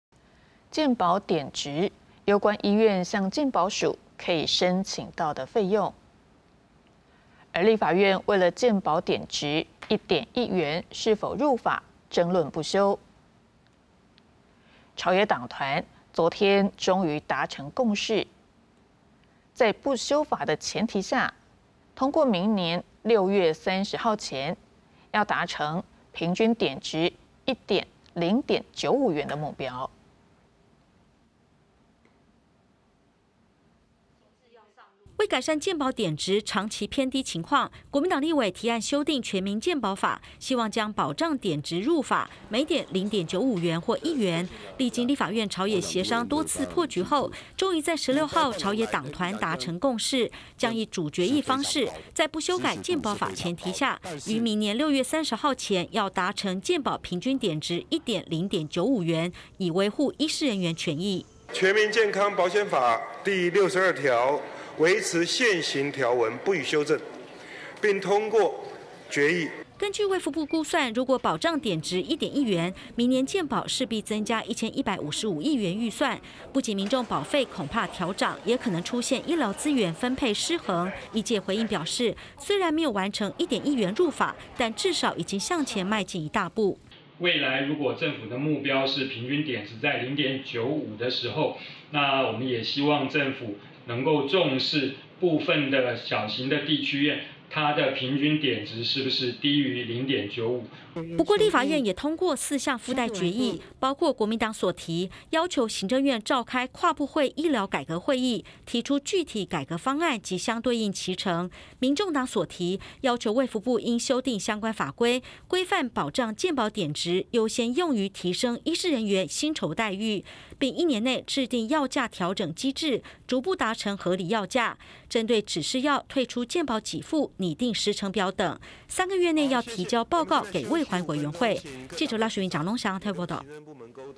健保點值通過主決議 明年6/30前達1點0.95元 | 20240717公視手語新聞
健保點值、攸關醫院向健保署可以申請到的費用。立法院為了、健保點值「1點1元」是否入法，爭論不休。朝野黨團、昨天終於達成共識，在「不修法」的前提下，通過明年6月30號前、要達成平均點值1點0.95元的目標。